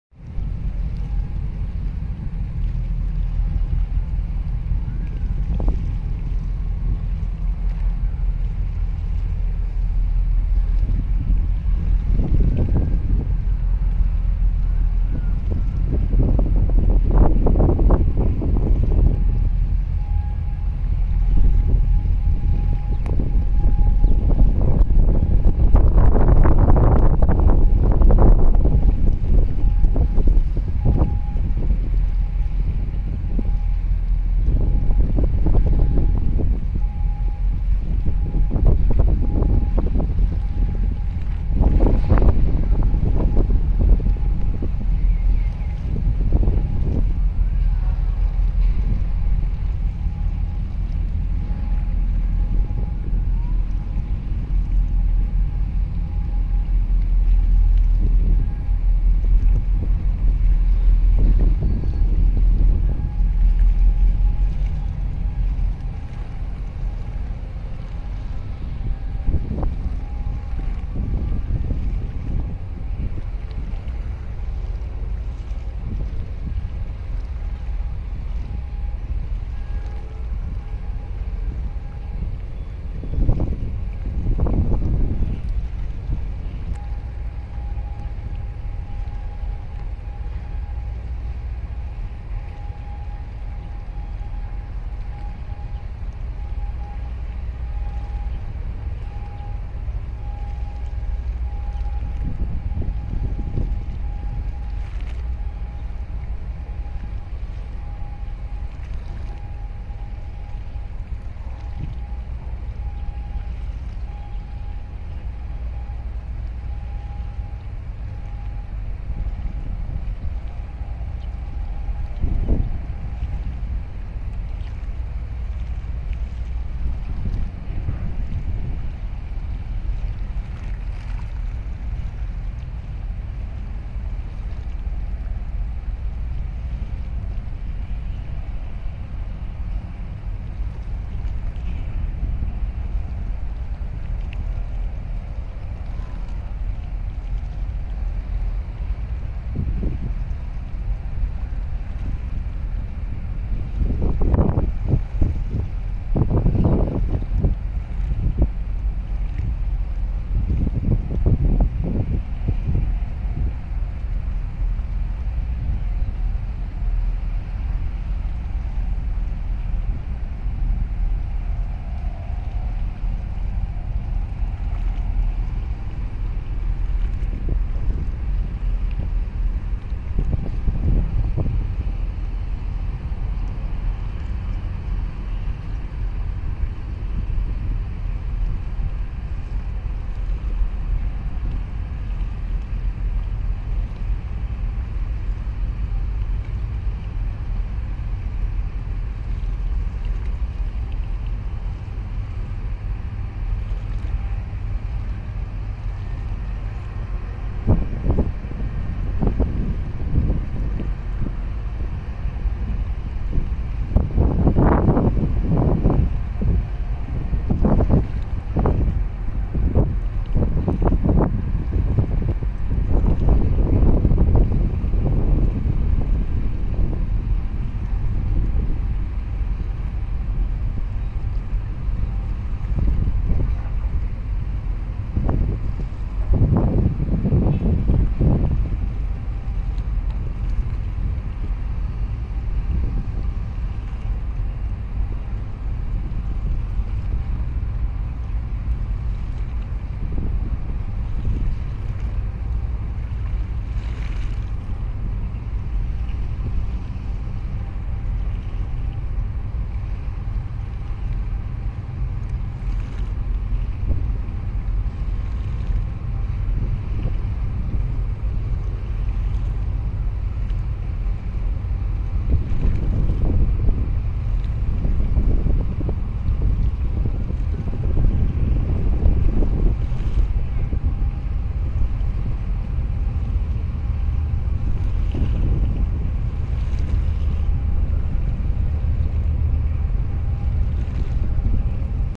BW CONFIDENCE LPG Tanker 液化石油氣體船 (4061浮筒→S2出港) 船籍英國UK
BW CONFIDENCE(BW GAS GROUP)退租改名NV AQUAMARINE

16:30分引水出發16:36分經過信號台
前次港口: 臺灣深澳港 (6/14)
6/15進二港停泊4061號浮筒6/16出港
出港前往: ECLLD  (6/24)

IMO 9307736
Ship typeLPG Tanker
Flag Isle of Man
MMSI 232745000
Gross Tonnage48772
Deadweight (t)54490
Length (m)230
Beam (m)36.6
Year of Built 2006
Builder MHI NAGASAKI SHIPYARD & ENGINE WORKS
Yard NAGASAKI